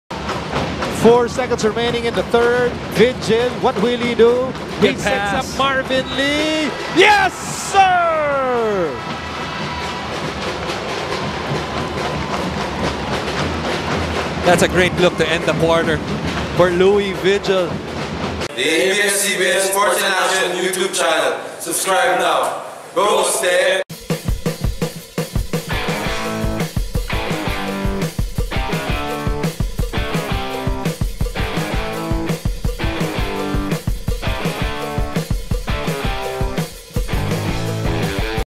Marvin Lee with a buzzer beater 3 pointer vs NU | UST | Best Plays | UAAP 79 - 2016
Watch Marvin Lee with a buzzer beater 3 pointer against National University.